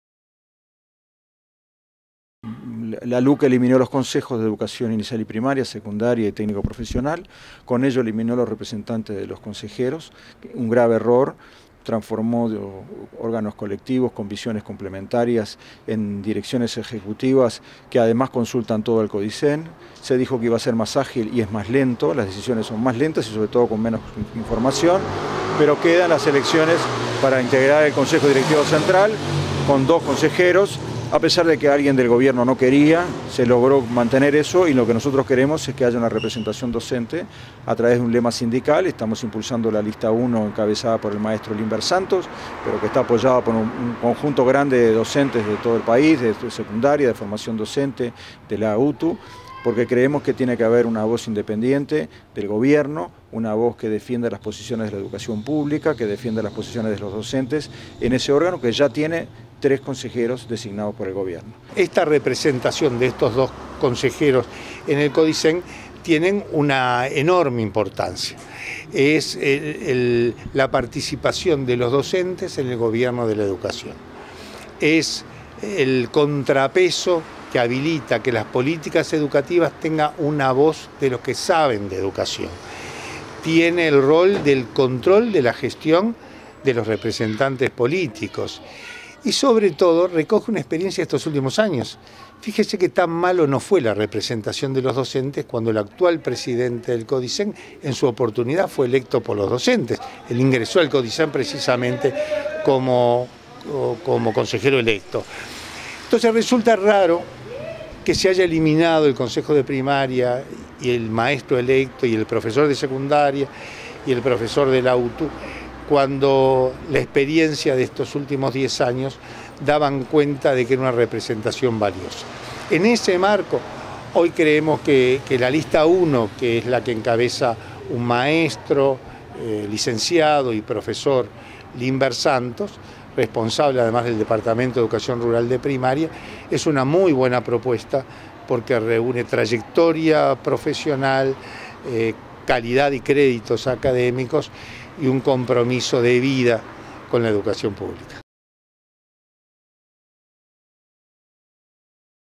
Elecciones del 30 de Octubre- Por dos cargos docentes en el consejo directivo central
Las elecciones para elegir a la representación docente en la ANEP serán el 30 de octubre; ese día también se eligen el representante por el orden estudiantil en el Consejo de Formación en Educación y los delegados para las ATD. Luis Garibaldi (ex Director de Educación) y Héctor Florit (Ex Consejero de Primaria), se refirieron a la instancia e invitaron a votar la Lista 1 encabezada por el profesor Limber Santos.